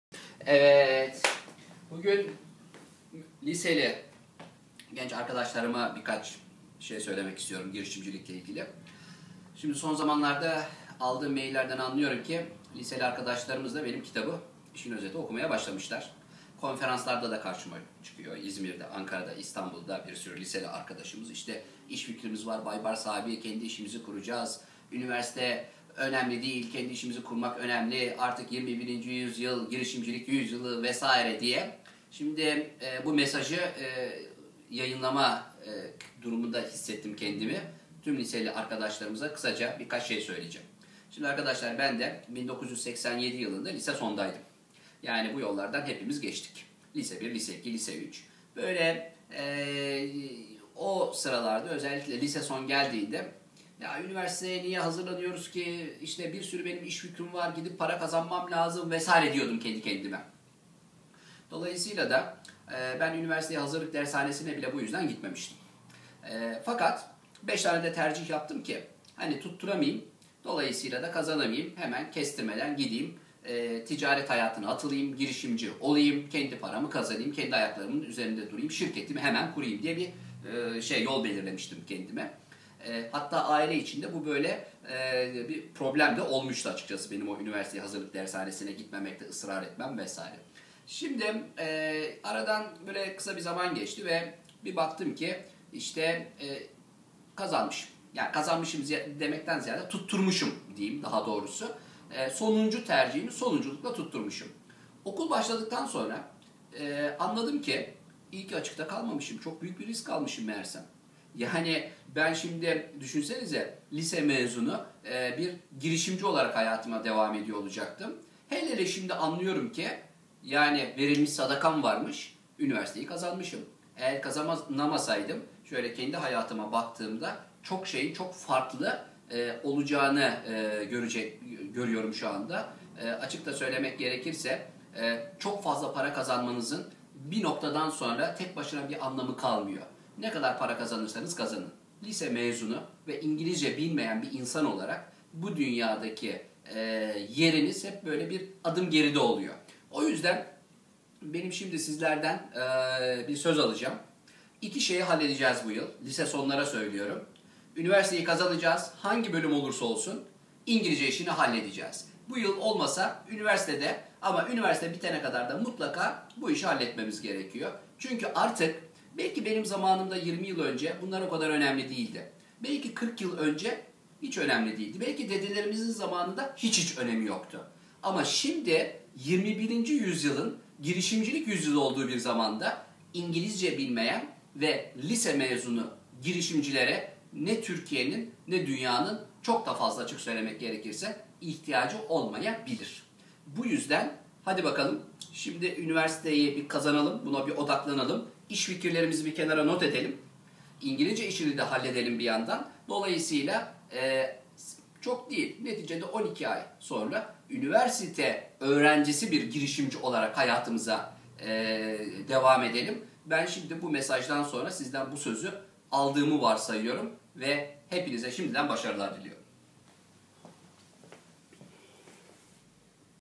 Girisimci Lise Ogrencilerine Dragon Abilerinden Cok Onemli
Liseli genc girisimcilere Baybars Altuntas'in cok onemli mesajini iceren kisa video.